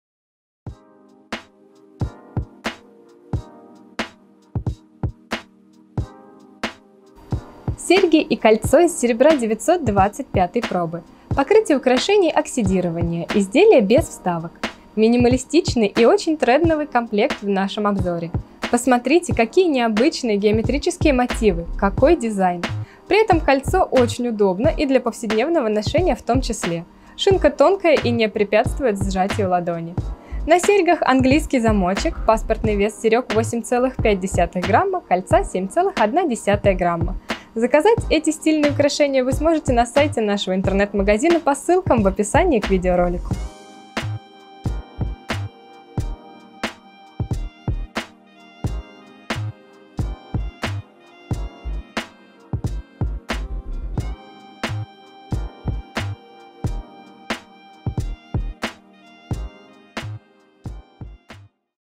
Серьги 33012810 и кольцо 23012810
Покрытие: оксидирование
Без вставок

На серьгах английский замочек
Паспортный вес серёг - 8,5 грамм, кольца - 7,1 грамм
Минималистичный и очень трендовый комплект в нашем обзоре! Посмотрите, какие необычные геометрические мотивы, какой дизайн! При этом кольцо очень удобно и для повседневного ношения в том числе - шинка тонкая и не препятствует сжатию ладони.

Заказать эти стильные украшения вы сможете  на сайте нашего интернет-магазина по ссылкам в описании к видеоролику.